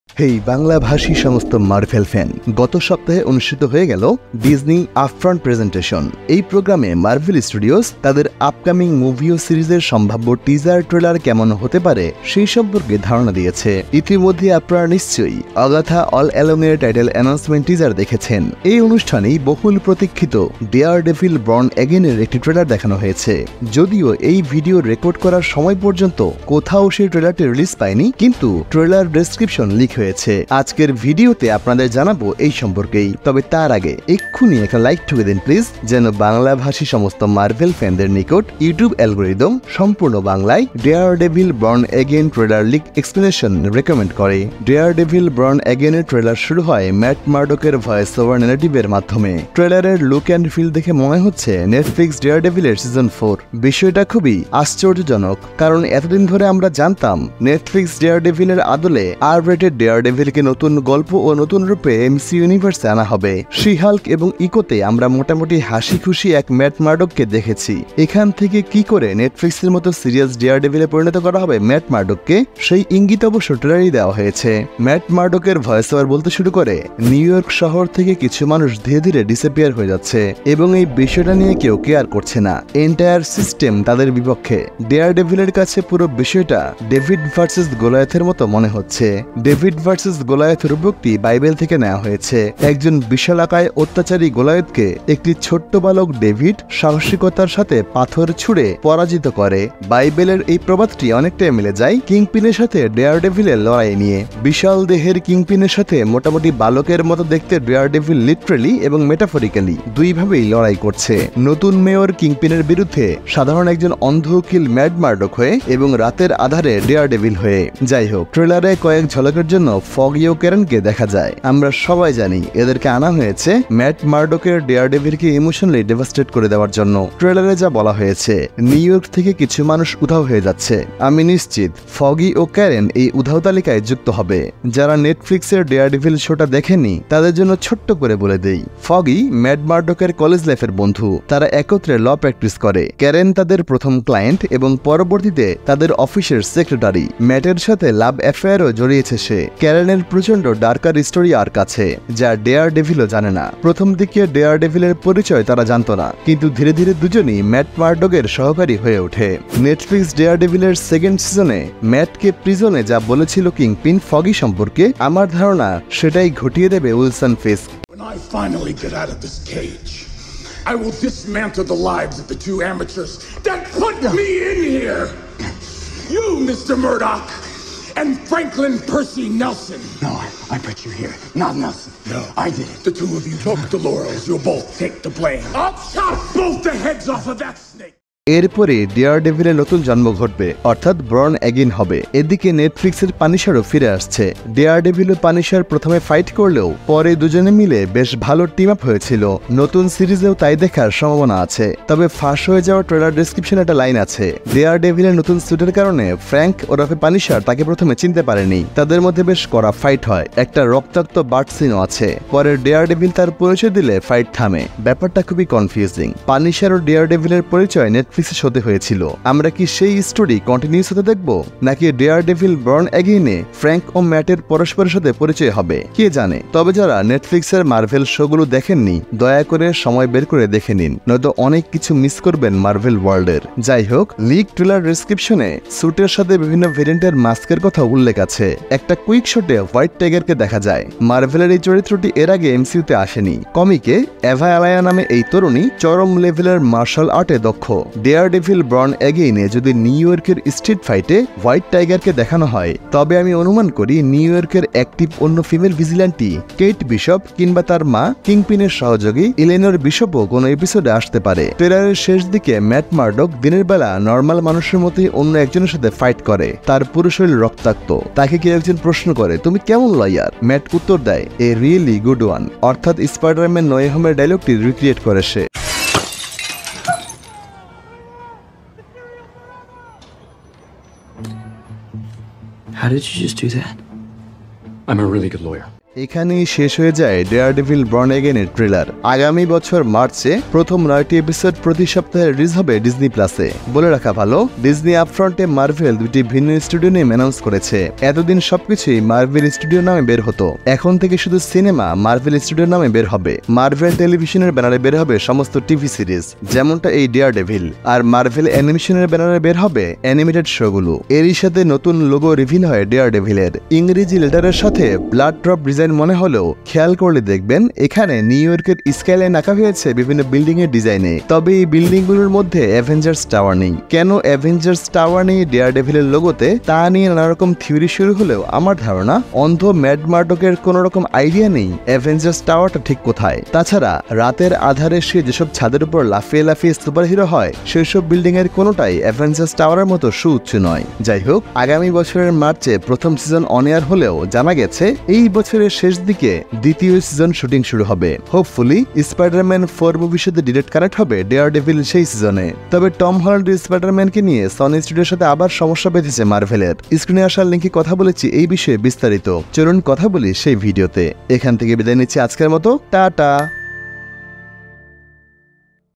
একসাথে আসছে Daredevil ও Punisher । Daredevil Born Again Leak Trailer Explained in Bangla
হেই বাংলাভাষী সমস্ত marvel fan, গত সপ্তাহে অনুষ্ঠিত হয়ে গেল Disney Upfront Presentation. এই প্রোগ্রামে Marvel Studios তাদের upcoming movie ও series এর সম্ভাব্য Teaser Trailer কেমন হতে পারে, সে সম্পর্কে ধারণা দিয়েছে। ইতোমধ্যেই আপনারা নিশ্চয় Agatha All Along এর Title Announcement Teaser দেখেছেন। এই অনুষ্ঠানেই বহুল প্রতীক্ষিত daredevil daredevilbornagain এর একটি Trailer দেখানো হয়েছে। যদিও এই ভিডিও রেকর্ড করার সময় পর্যন্ত কোথাও সেই Trailer টি রিলিজ পায় নি, কিন্ত Trailer Description Leak হয়েছে। আজকের ভিডিওতে আপনাদের জানাবো এই সম্পর্কেই।

©Filmy_Bangladeshi